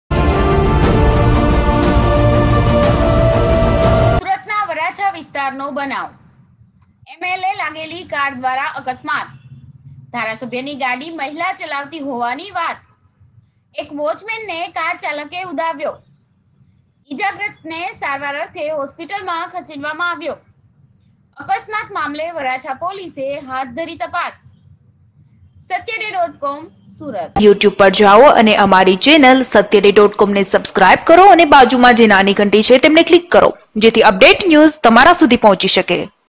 સુરત એમ.એલ.એ. ની કાર બની યમદૂત.....જુઓ વિડીયો...
સૂરત બ્રેકીંગ

સુરત ના વરાછા વિસ્તારનો બનાવ..

એમ.એલ.એ લાગેલી કાર દ્વારા અકસ્માત..

ધારાસભ્યની ગાડી મહિલા ચલાવતી હોવાની વાત..

એક વોચમેન ને કાર ચાલકે ઉડાવ્યો.

ઇજાગ્રસ્ત ને સારવાર અર્થે હોસ્પિટલ માં ખસેડવામાં આવ્યો.

અકસ્માત મામલે વરાછા પોલીસે હાથ ધરી તપાસ.